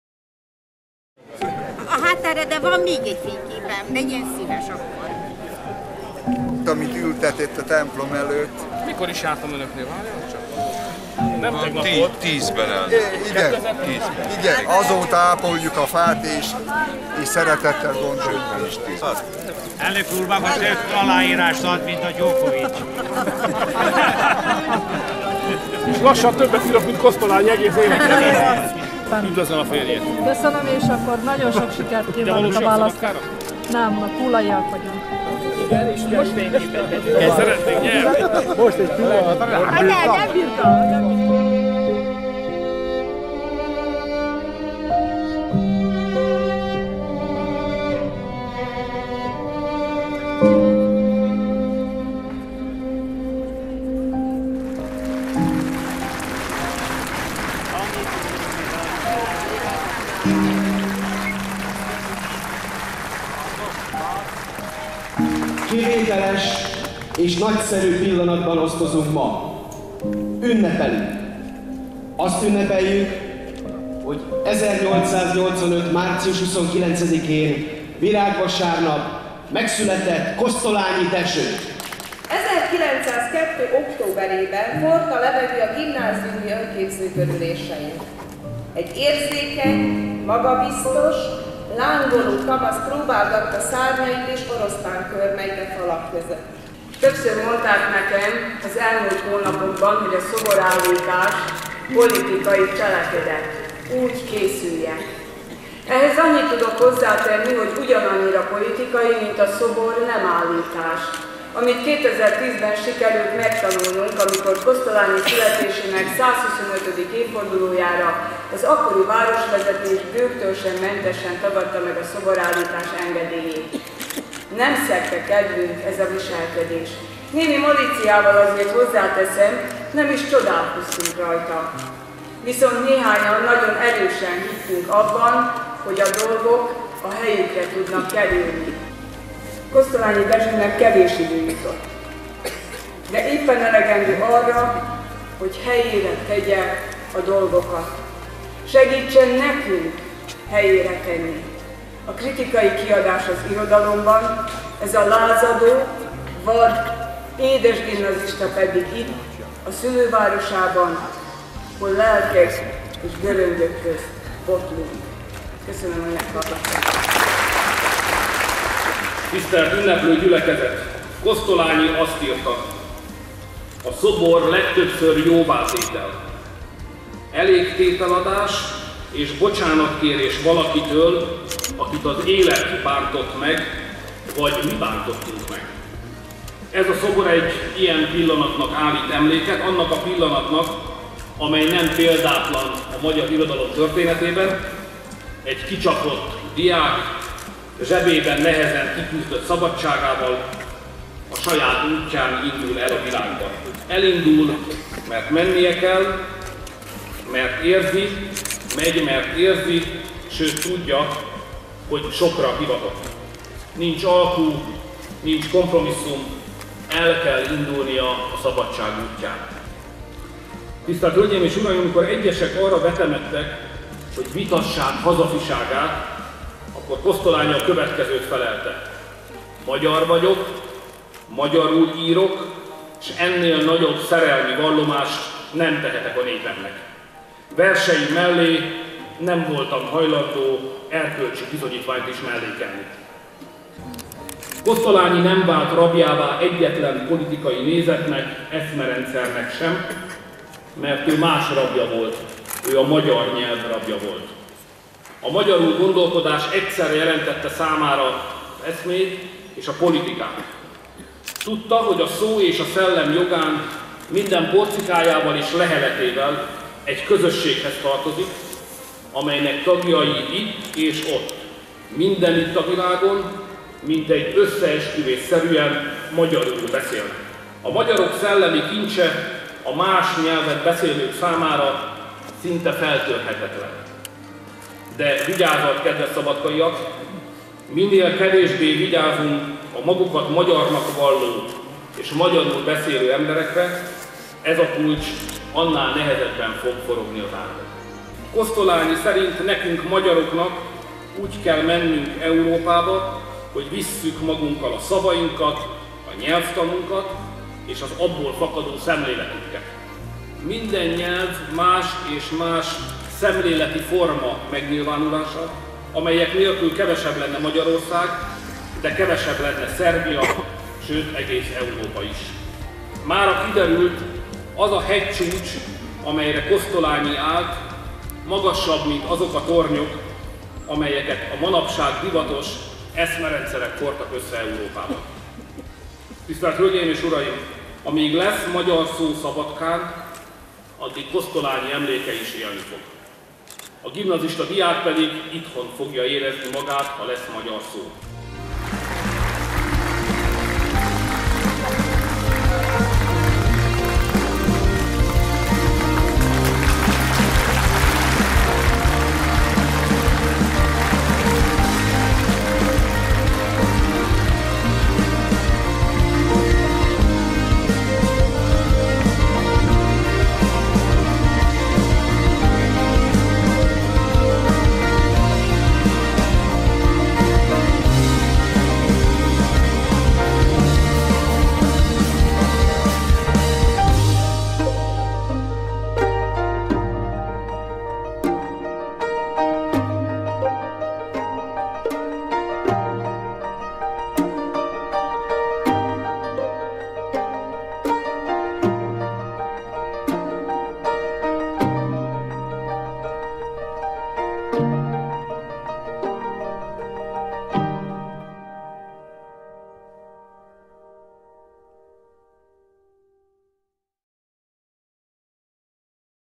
A Kosztolányi-szobor avatóünnepsége Szabadkán
Felavatták Szabadkán A gimnazista nevet viselő bronzszobrot, amelyet a város szülöttének, Kosztolányi Dezsőnek a 129. születésnapja alkalmából adtak át. Az alkotás Szarapka Tibor szobrászművész munkája, és egy, a 17 éves Kosztolányi Dezsőről készült fotó alapján készült. Orbán Viktor, Magyarország miniszterelnöke a Svetozar Marković Gimnázium két tanulójával közösen leplezte le a közel két méteres szobrot. A magyar kormányfő Pásztor István meghívására érkezett Vajdaságba, hogy részt vegyen a Kosztolányi Napok rendezvényein. A Kosztolányi Dezső-szobor felállítását az Magyar Nemzeti Tanács kezdeményezte, a magyar kormány pedig jelentős összeggel támogatta.

2014. március 28.